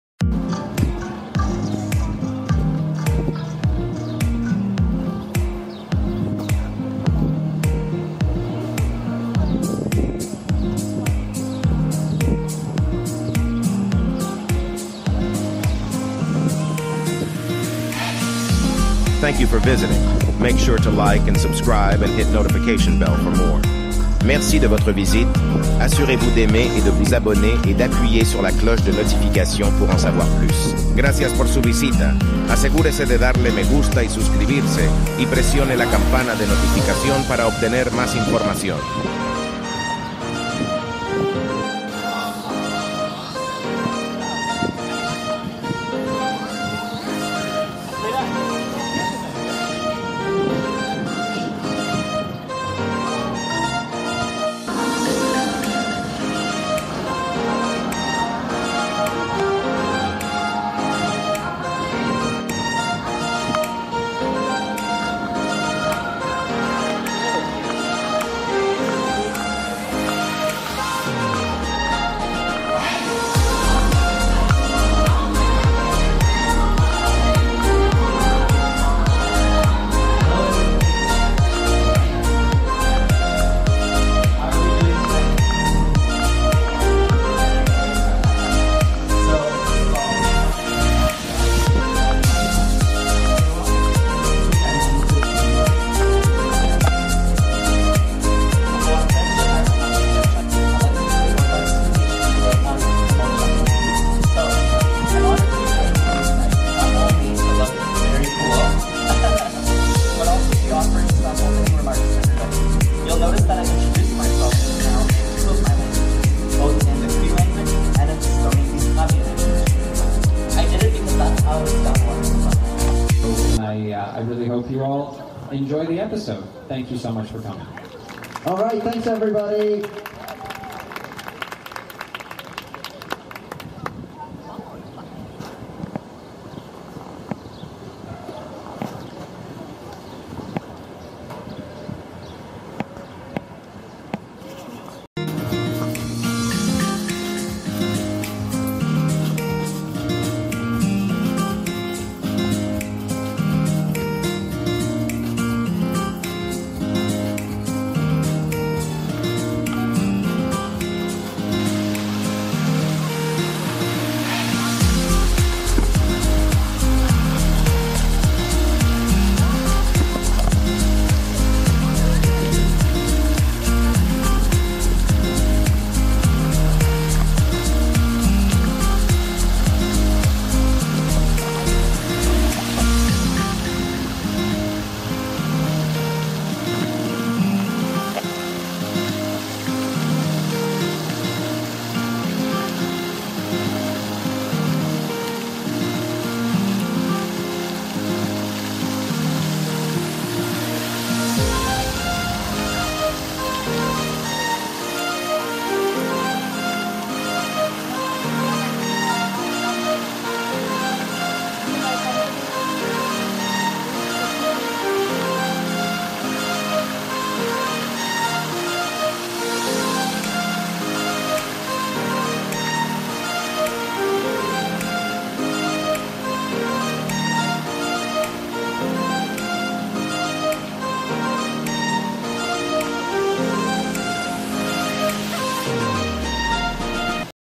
PRIME MOVIE NIGHT: THE LAKE WITH JORDAN GAVARAIS - COLLEGE PARK TORONTO
JUNE 29 2023 - AMAZON PRIME presents MOVIE NIGHT at College Park in Toronto.  In presence is Jordan Gavarais starring in THE LAKE, afterwards they shown the classic movie starring the late Robin Williams BIRD CAGE.

AMAZON PRIME présente MOVIE NIGHT au College Park de Toronto. En présence, Jordan Gavaris joue dans THE LAKE, après quoi ils ont montré le film classique mettant en vedette le regretté Robin Williams BIRD CAGE.

AMAZON PRIME presenta MOVIE NIGHT en College Park en Toronto. Presencia Jordan Gavaris protagonizando THE LAKE, luego proyectaron la clásica película protagonizada por el fallecido Robin Williams BIRD CAGE.